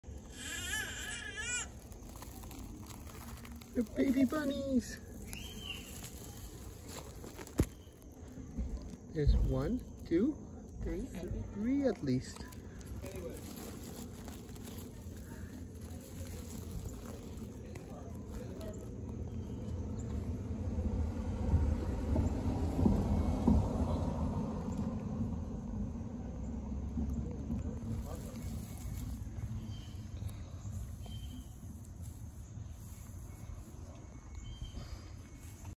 Baby bunnies
The kids in the neighborhood found a hole with baby bunnies on a sidewalk